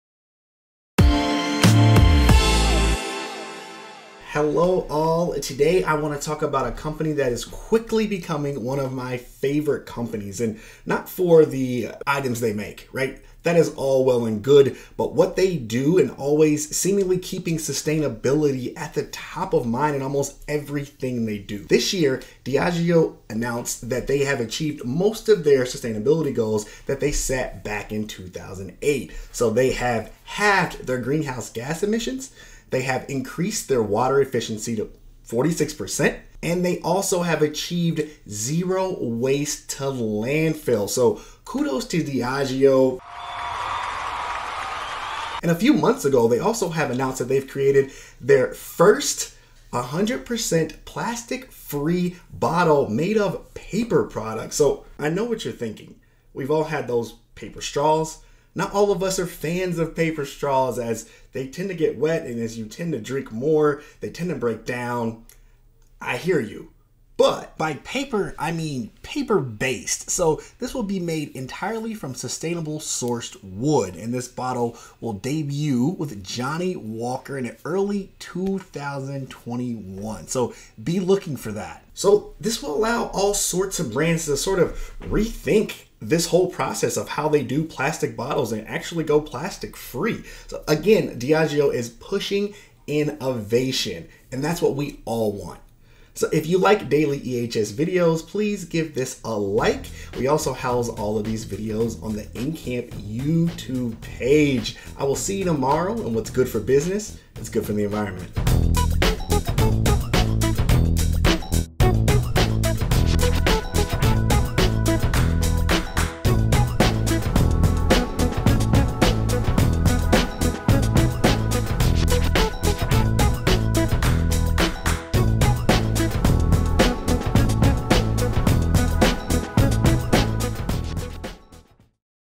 Diageo Adds a 100% Plastic-Free Spirits Bottle
An EHS Moment: Diageo announced in July they are releasing the first-ever 100% plastic-free spirits bottle, made entirely from sustainably sourced wood. The bottle will debut with Johnnie Walker

Diageo, the spirits company, the maker of brands like Johnny Walker, Guinness, and Smirnoff is quickly becoming one of my favorite companies for their industry-leading environmental goals and achievements. Kudos!

Sustainability goals met:
✅ Cutting greenhouse gas emissions across direct
operations in half since 2008

✅ Improved water efficiency by 46%

✅ Achieved zero waste-to-landfill at all production sites and
offices